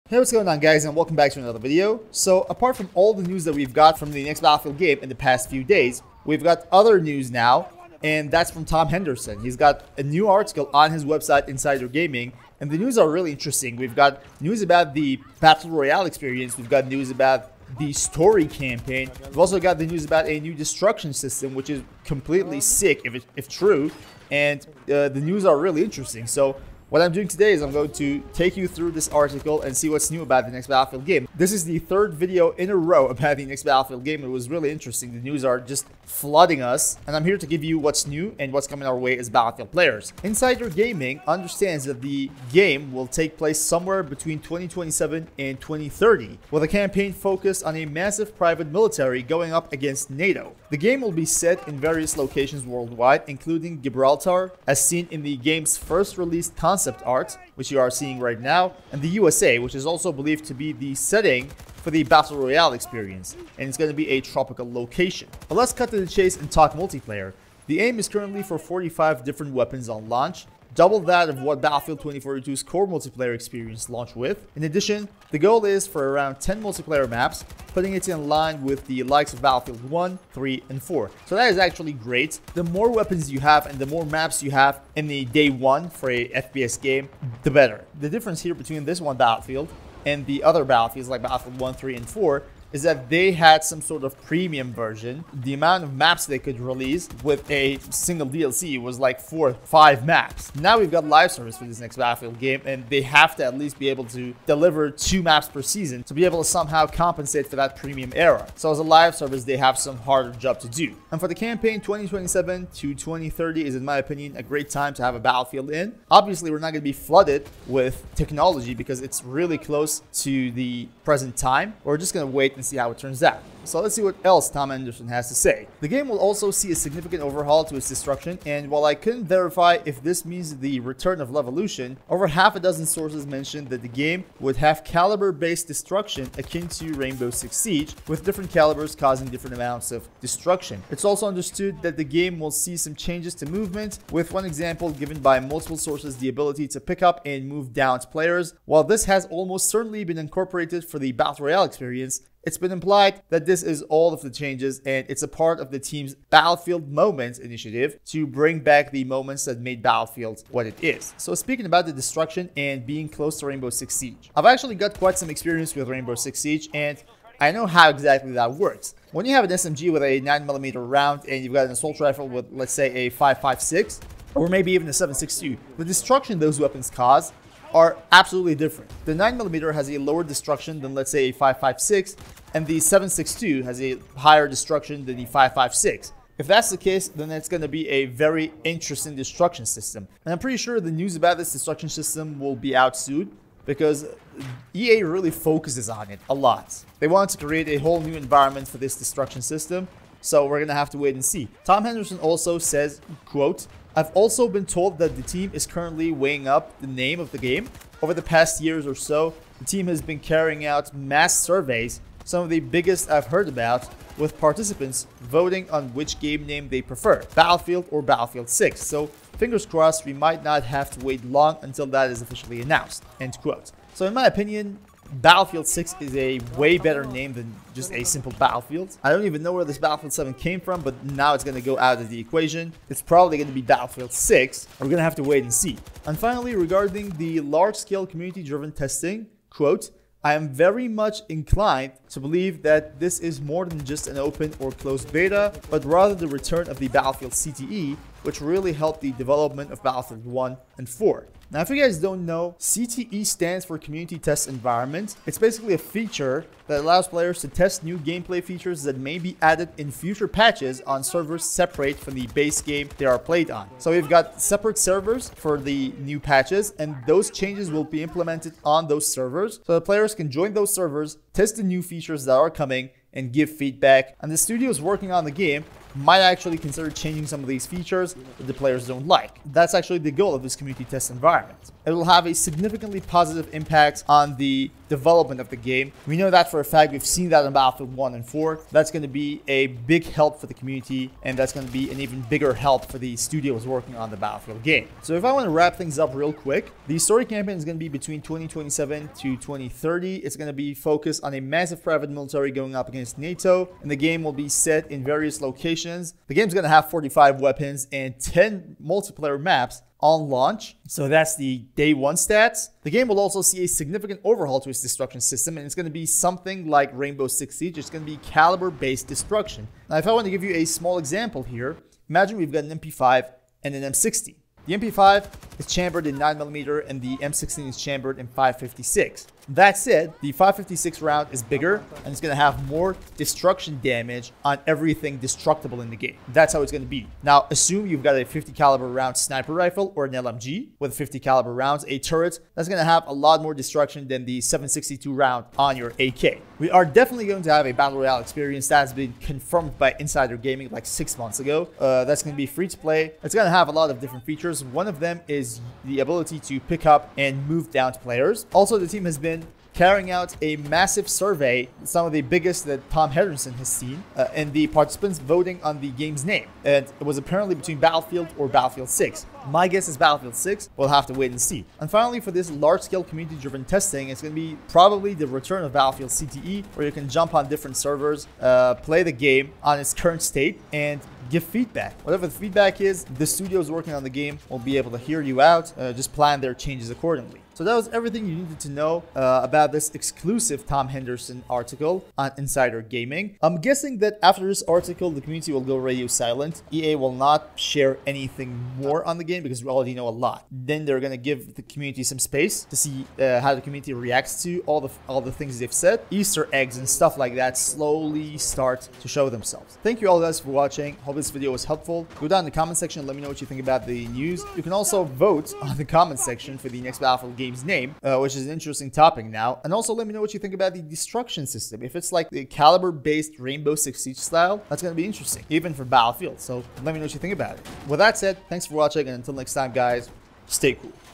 Next Battlefield Game Features A "New Destruction System" + A Lot of Weapons!
The next Battlefield which is probably gonna be called Battlefield 6, will have a lot of what Battlefield community has been craving for the past decade! A new destruction system is only one. This game's gonna have 45 weapons at launch and 10 multiplayer maps to begin with! This amount of weapons is Double the amount that Battlefield 2042 had at launch which is a great leap! A new Battle Royale experience is also on the way!

00:00 Intro
00:40 The Story Campaign
01:05 Weapons and Maps
02:05 Destruction and Movement
03:21 Next Game's Name!
03:55 The Playtest Program
04:55 A Summary